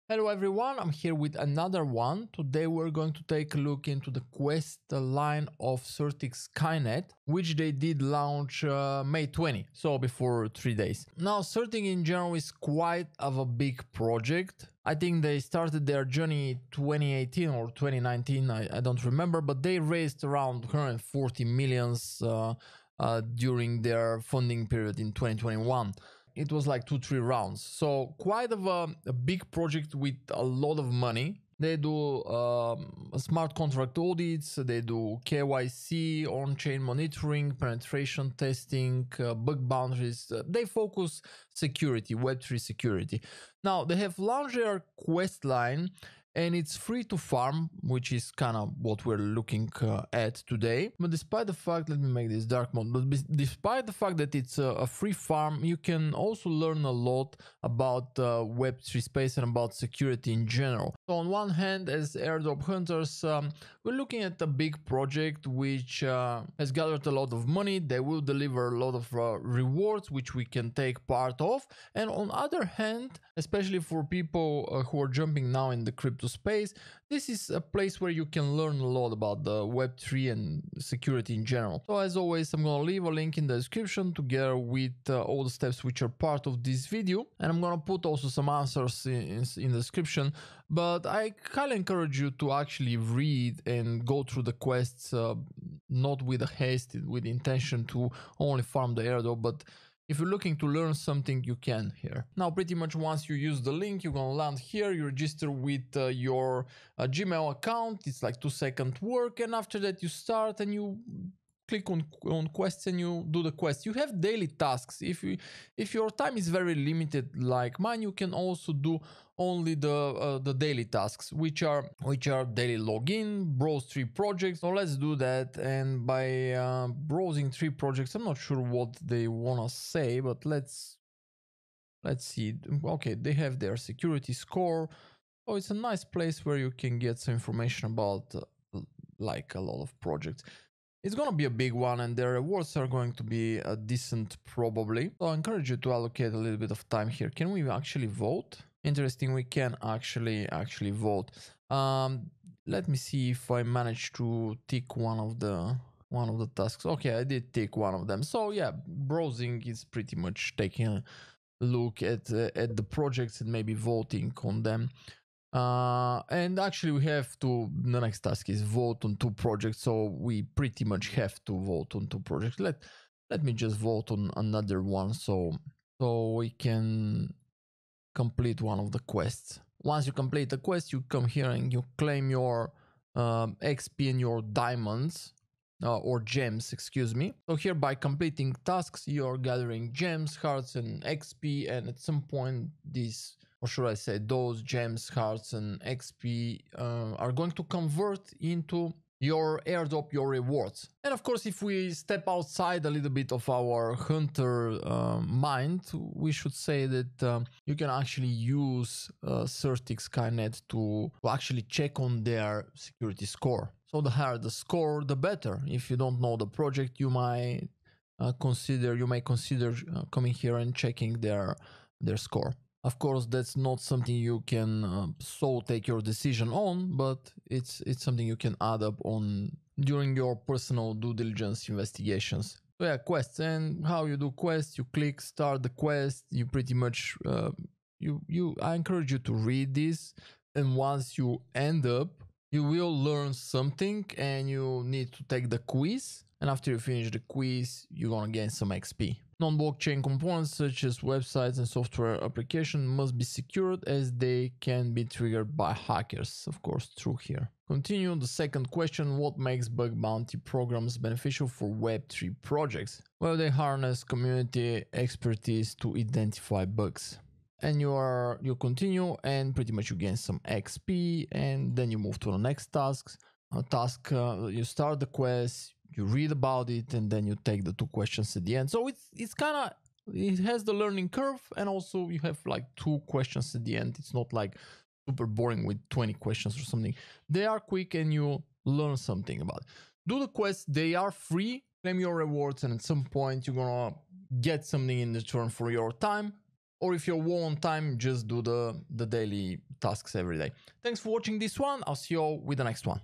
CeriK Skynet Questline Airdrop Guide
Certik Skynet are introducing their quest to earn campaign. Its free and you can earn some crypto rewards by doing the quests on Certik Skynet's platfrom.

Certik Skynet Airdrop Steps:

- Register with gmail and thats it

2) Click on quests and do the quests.
- You can only do daily tasks like; daily login, browsing projects, voting on projects, etc. Watch the video.

3) First quest answers:
1 - Security scores
2 - False

4) Don't forget to claim your gems, hearts and xp.
- Complete as many Certik quests as you can to earn more xp